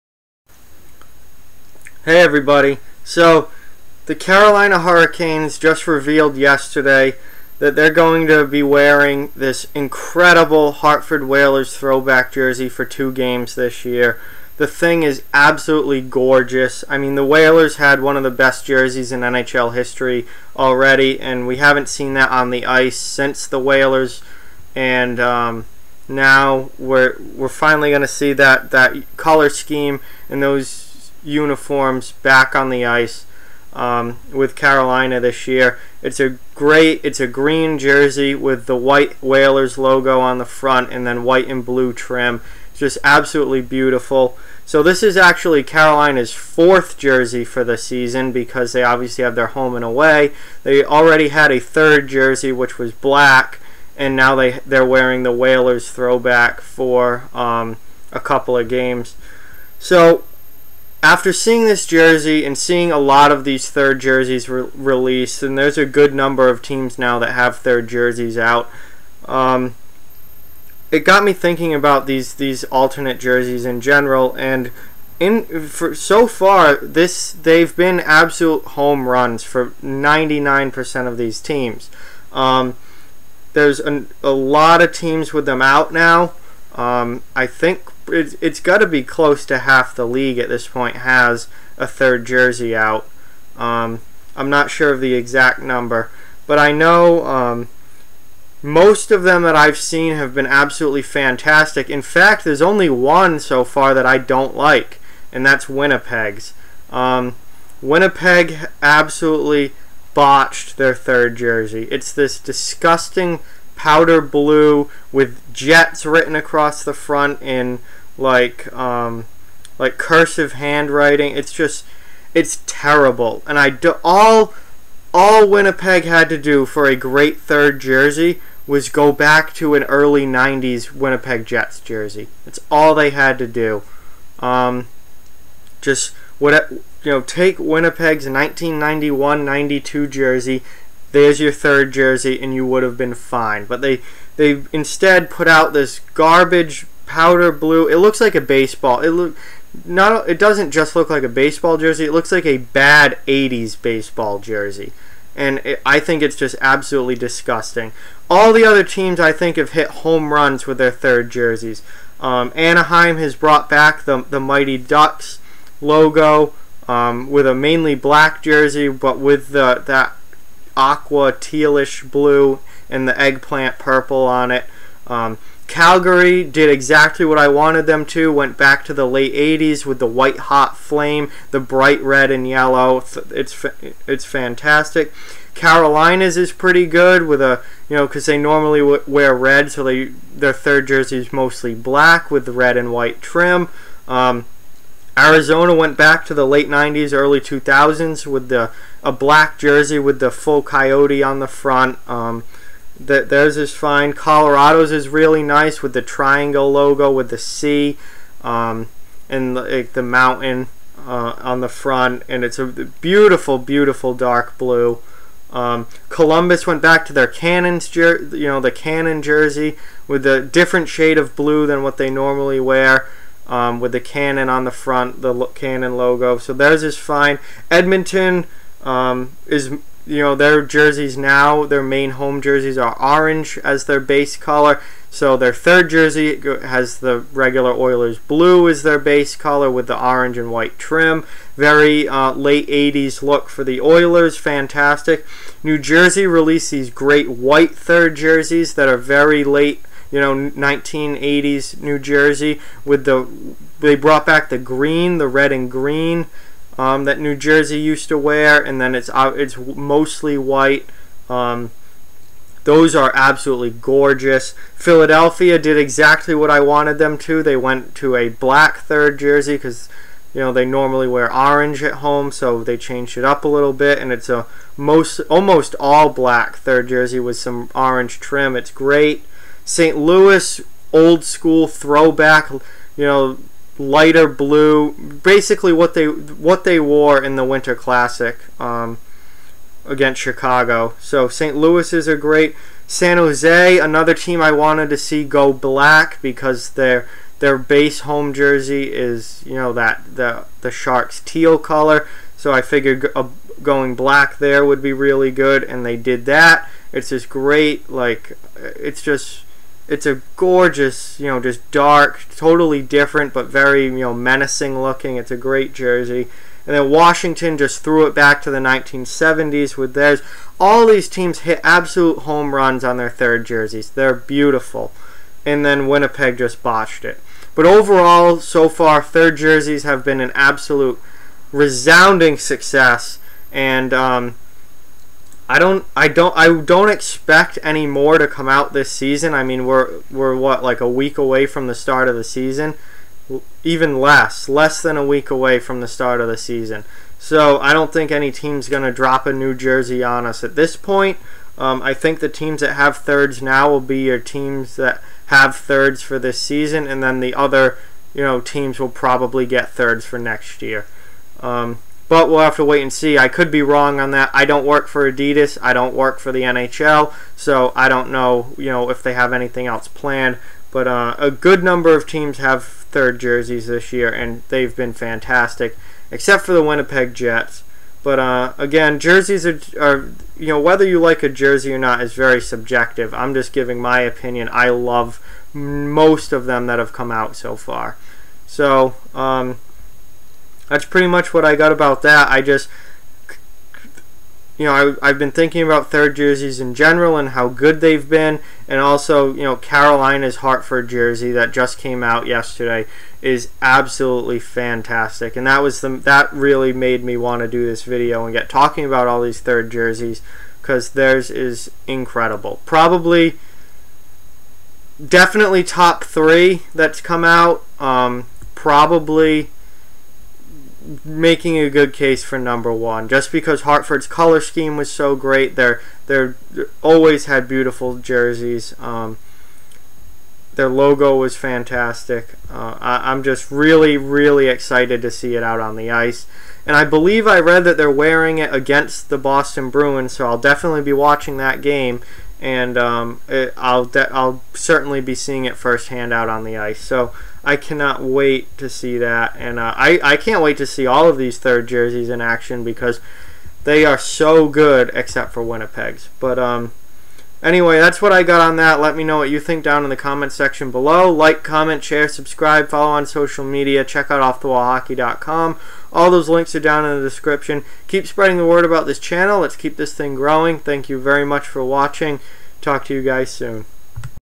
The Carolina Hurricanes Unveil Hartford Whalers Throwback Jersey
The Hurricanes will wear a Hartford Whalers throwback jersey for two games this year, and it is gorgeous